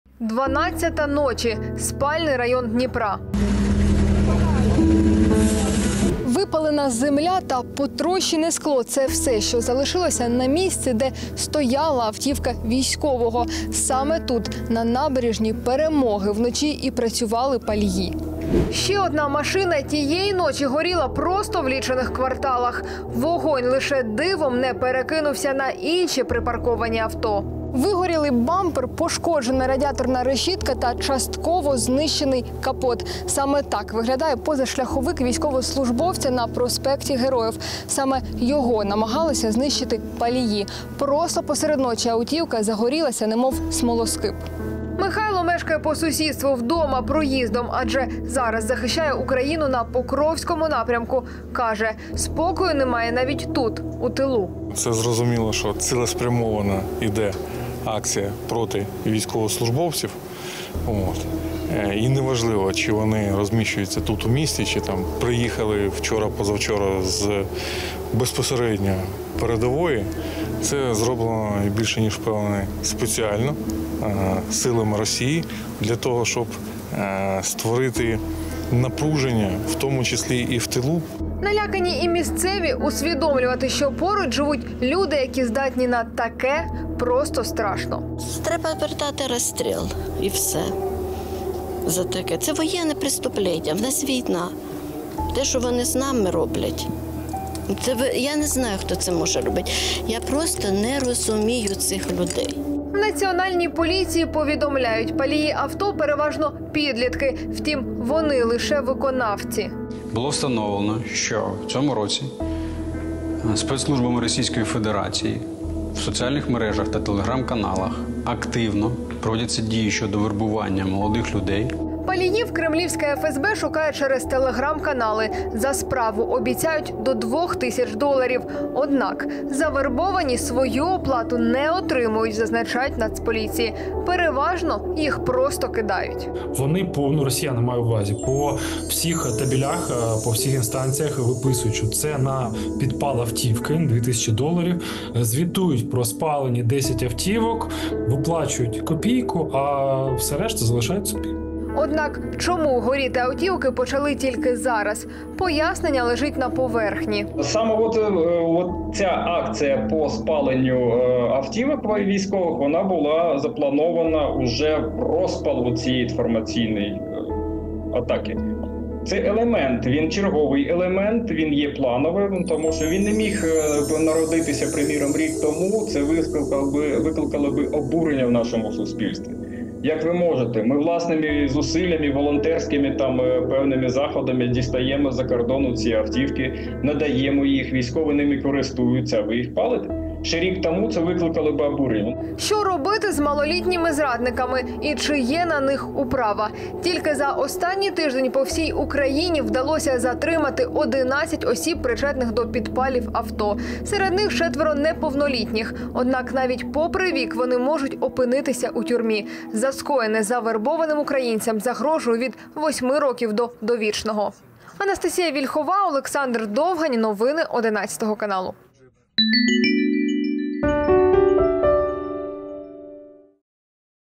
🔥Від 8 років до ДОВІЧНОГО. За тиждень - десятки затриманих паліїв автівок військових!
Чотири авто за три дні! У Дніпрі масово палять машини військових. Така невтішна статистика і загалом по Україні. Лише за минулий тиждень нацполіція разом зі співробітниками СБУ викрила 11 паліїв. Наразі відомо, вербує українців - російська агентура. За підривну діяльність працівники спецслужб обіцяють щедру грошову винагороду. Чому почастішали випадки? Як реагують військові? Та чи дійсно піромани можуть опинитися за ґратами довічно?